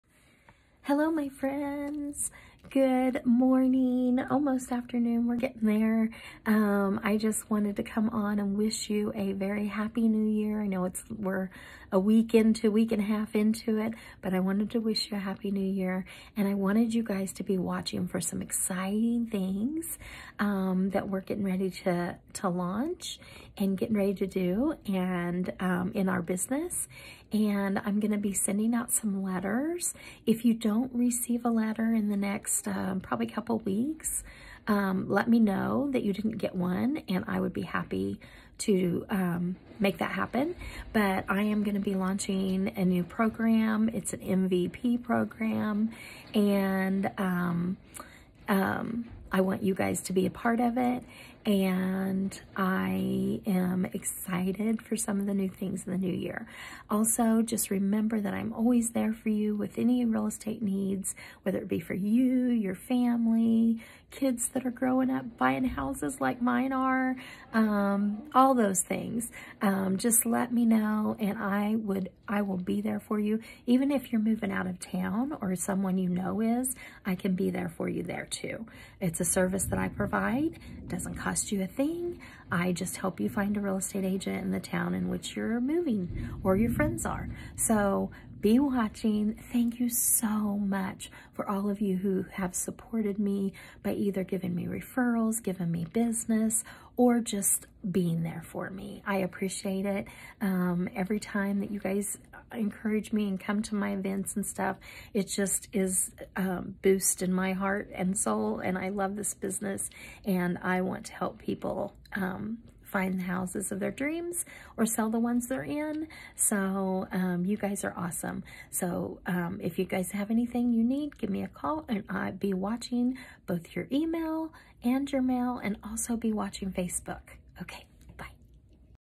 New things are coming!
Happy new year. A new year usually means new changes and I am going to be introducing you to some changes in my business. I will be launching a new MVP program. Still working out the details but I am excited.